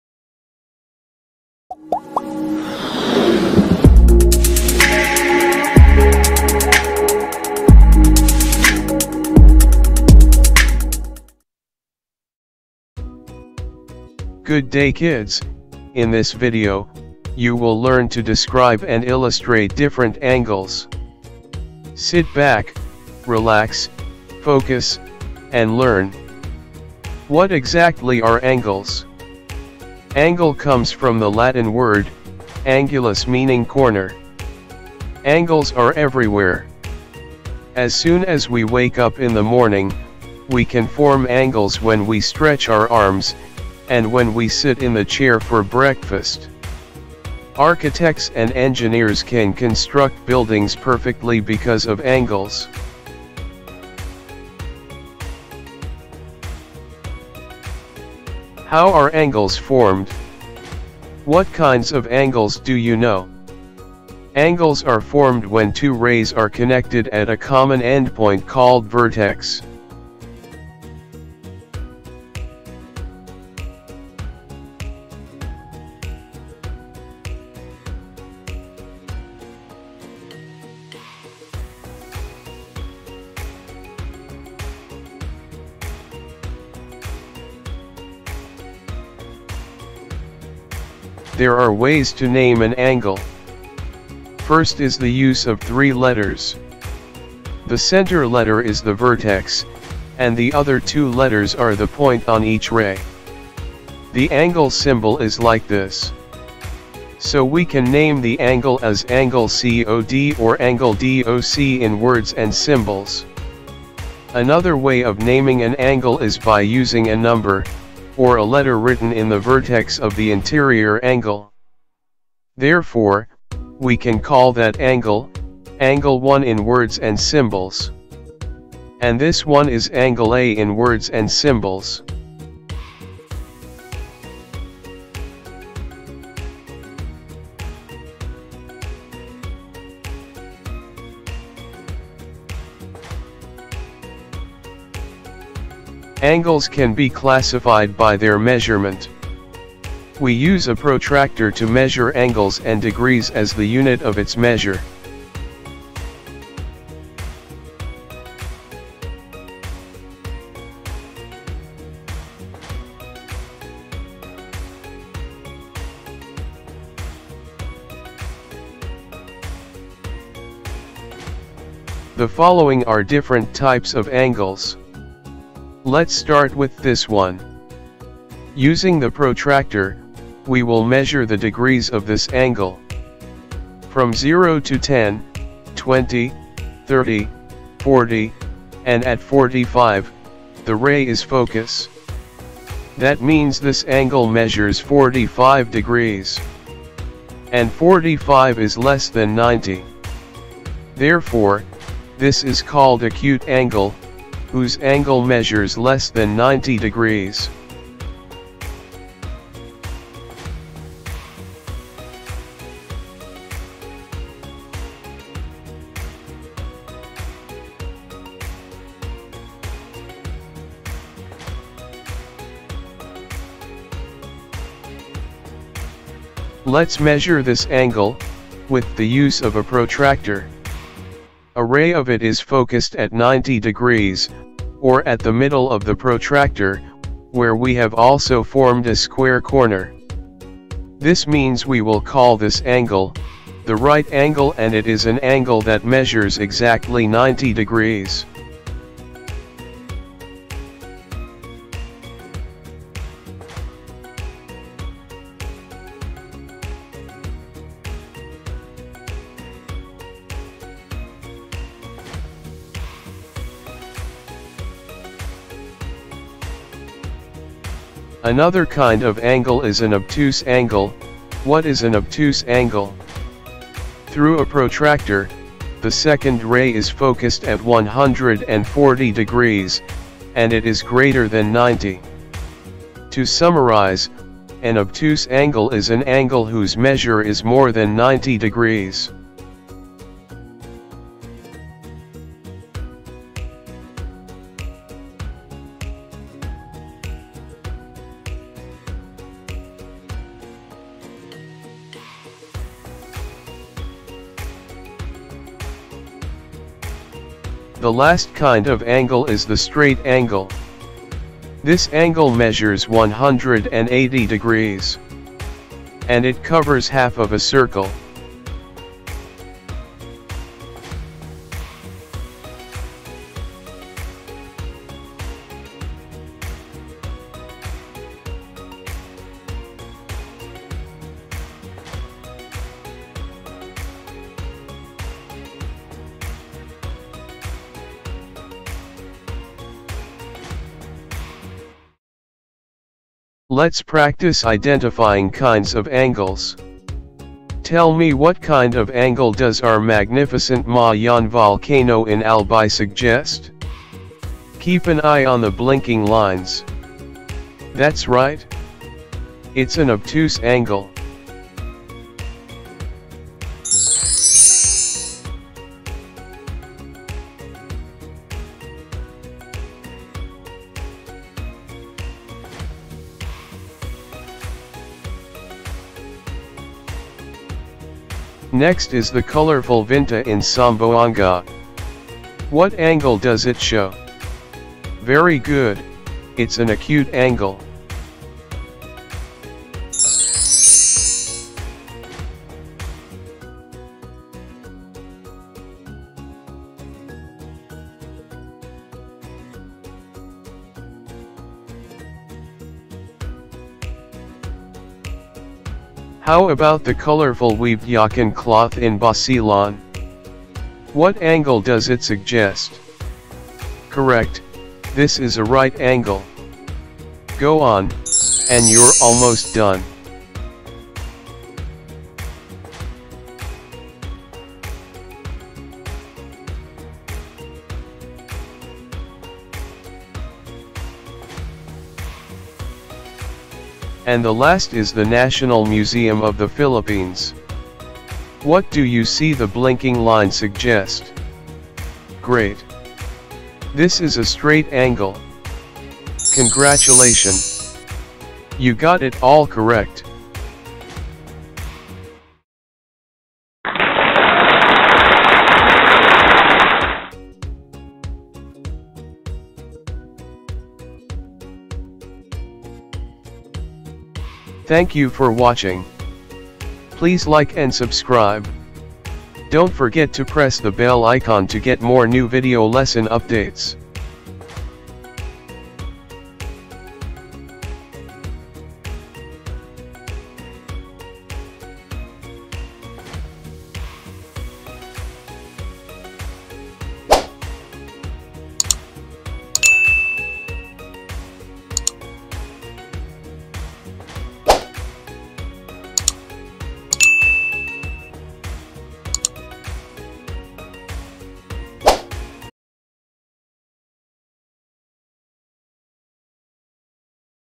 Describing and Illustrating Different Angles
In this video you will learn how to describe and illustrate different kinds of angles.

Its all about Mathematics and gaming videos for Kids.